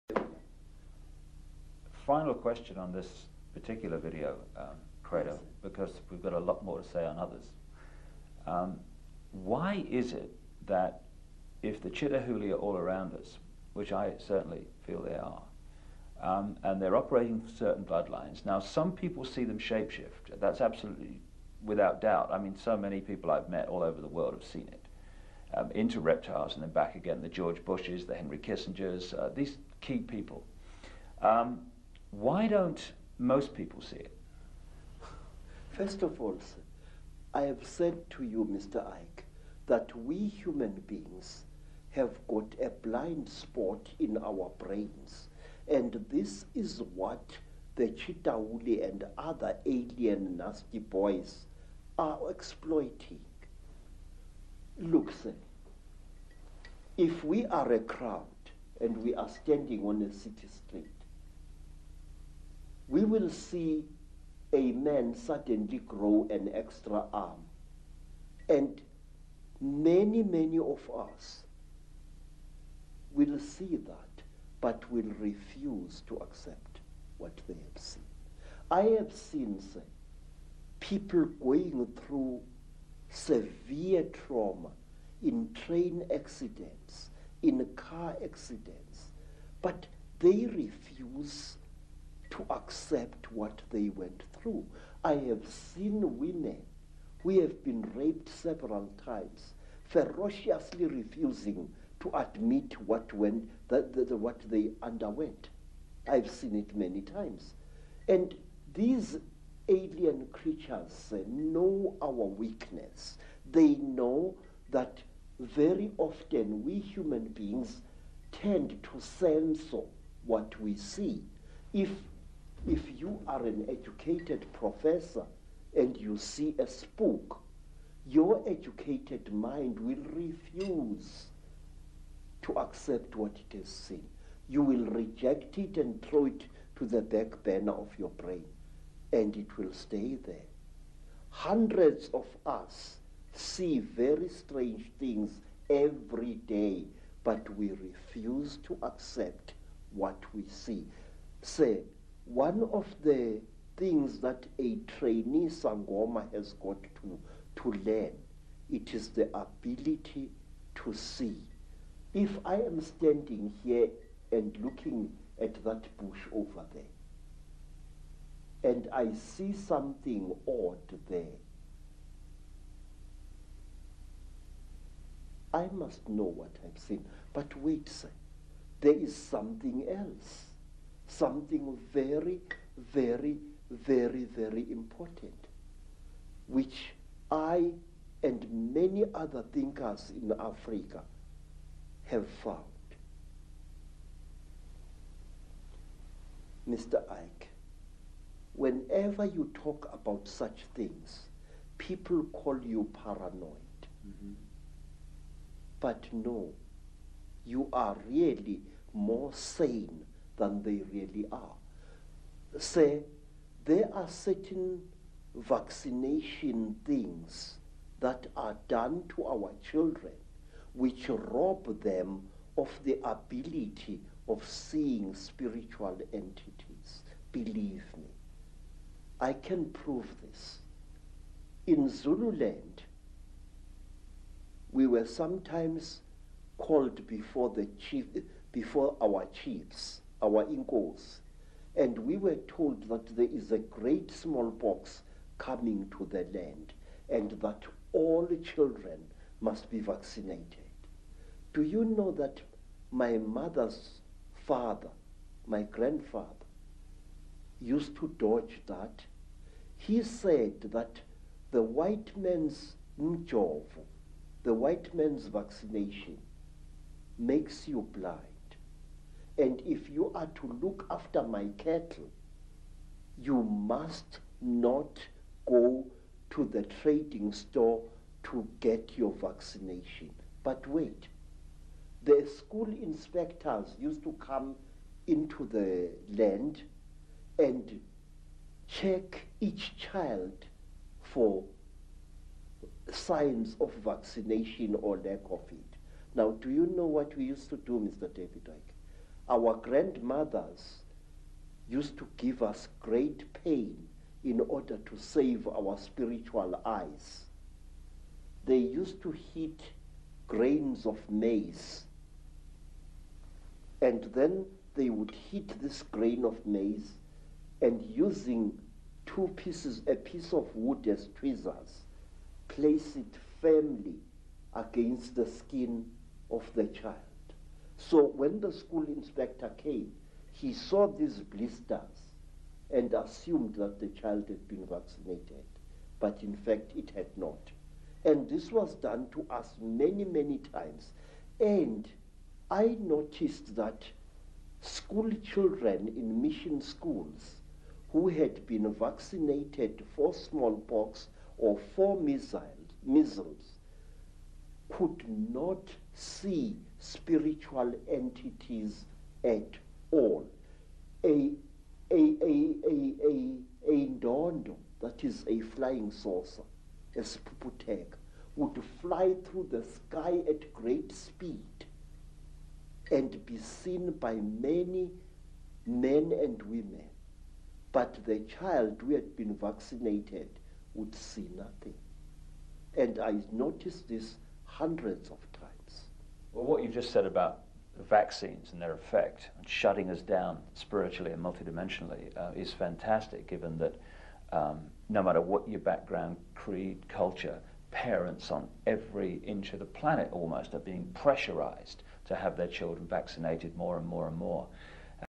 Credo Mutwa-17/18-Folks blindhet og vaksiner
- Norsk tekst, men beklageligvis meget dårlig gramatikk.
Credo Mutwa er Zulu shaman og født i 1921. Han bestemte seg i ung alder for å vie livet sitt til å ta vare på gammel kunnskap om Afrikas befolkning og kultur før den dør ut. Han forteller om krevende tester han måtte gjennom for å vise at han var helhjertet for oppgaven og han forteller ganske detaljert en helt annen historie enn hva historiebøkene gjør